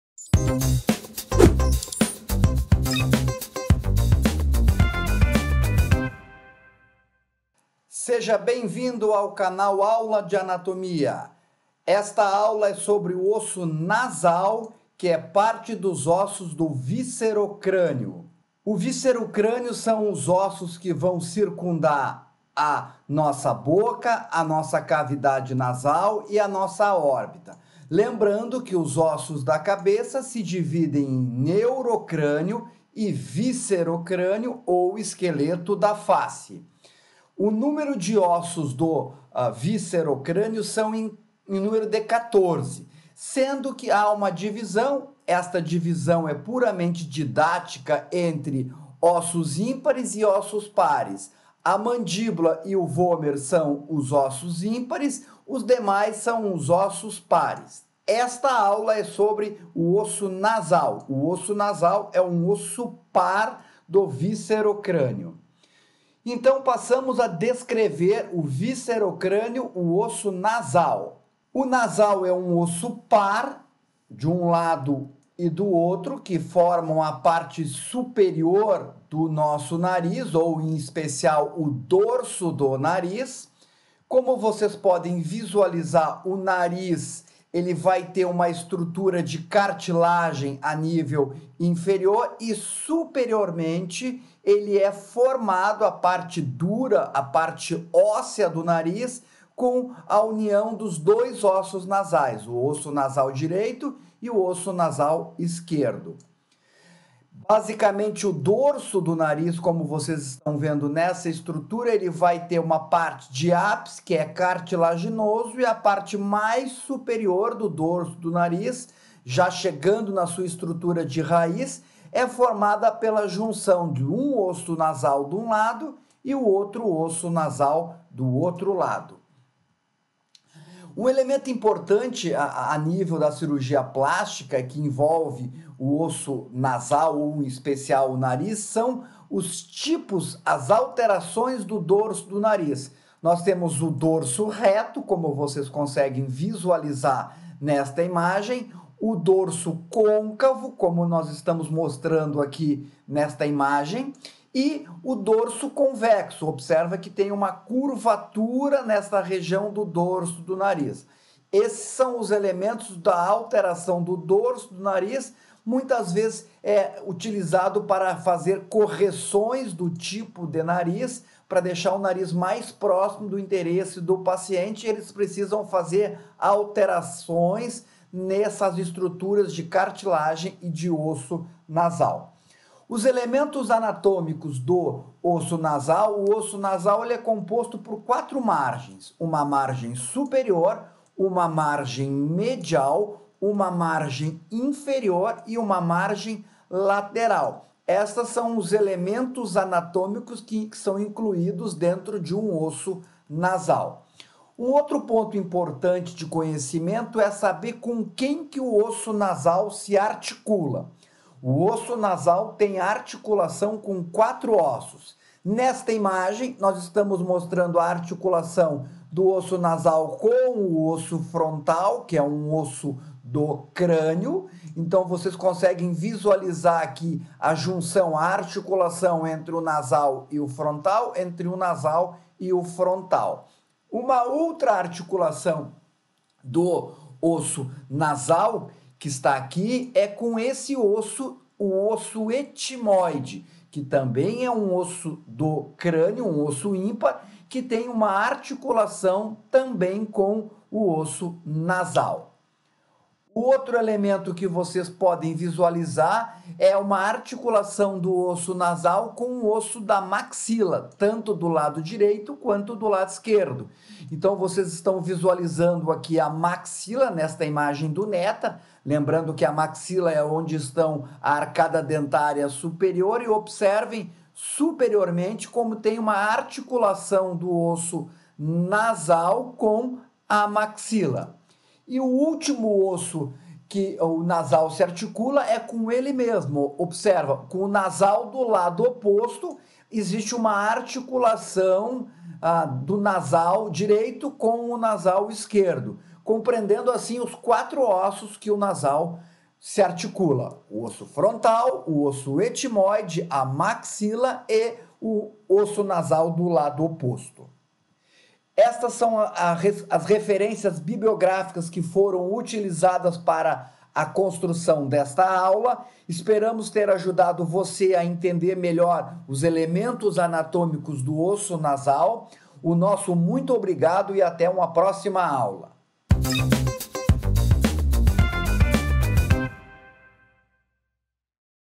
OSSO NASAL
Os ossos nasais são dois ossos pequenos e simétricos da face (crânio), que constituem a ponte do nariz. Seus limites superiores e corpos formam a ponte do nariz (glabela). Os limites inferiores se conectam com a cartilagem nasal para formar a margem superior da abertura nasal.